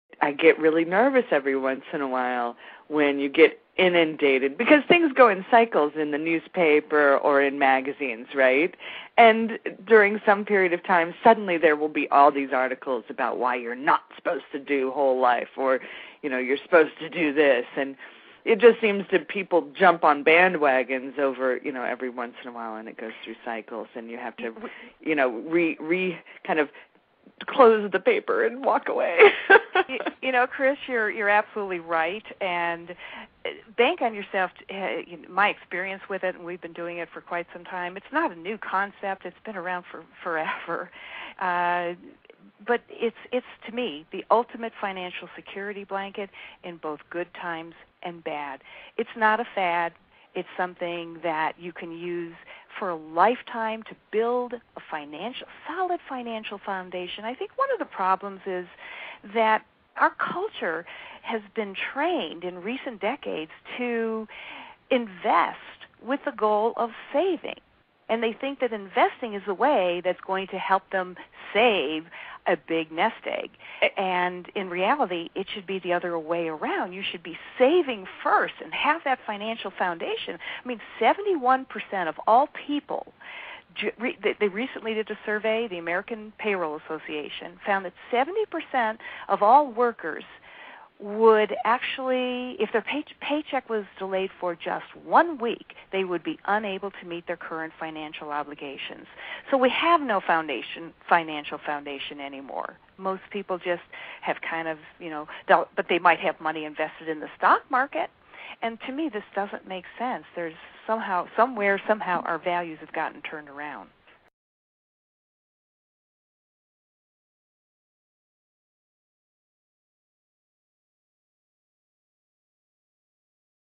Bank on Yourself | An Interview with Chicke Fitzgerald Pt. 6
Pamela Yellen, best-selling author of The Bank On Yourself Revolution, discusses the difference between Bank On Yourself and conventional methods of financial planning in an interview with Chicke Fitzgerald.

For many, the investment alternative described in Bank On Yourself may sound like the opposite of how Americans typically approach saving money. Most people assume the only way to grow significant wealth is to invest in the stock market or real estate with the goal of building a nest egg. What Pamela suggests is that saving and creating a solid financial foundation should come first. This might sound impossible, but with Bank On Yourself, it's quite easy to get started.

Many investment alternatives have come and gone over the years, but Bank On Yourself is not a trend or even a new concept. It's the ultimate financial security blanket for good times and bad, and it's available to almost anyone.